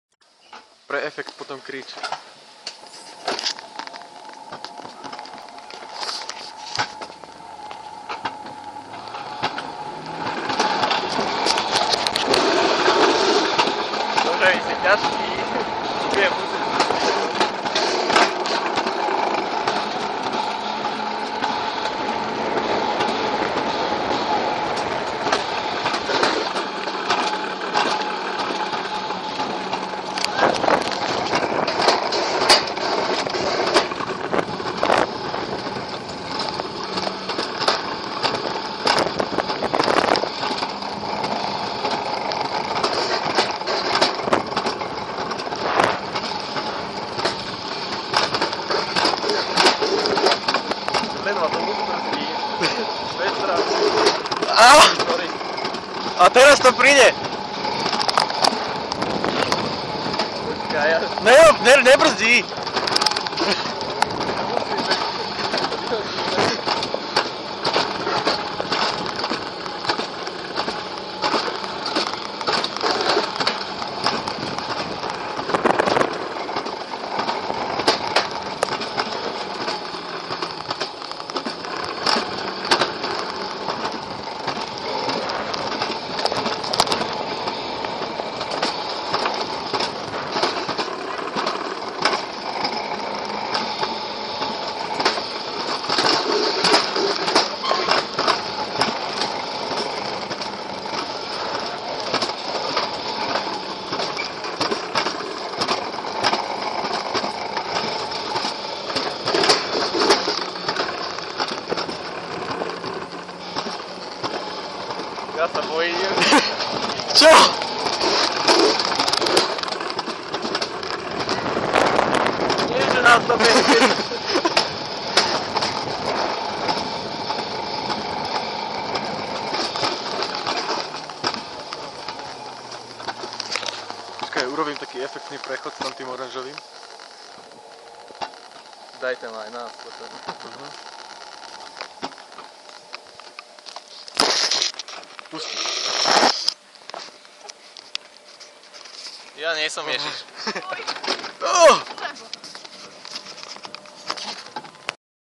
Bobovka - Velka Raca
Velka prca na Velkej Raci na bobovke s mastrom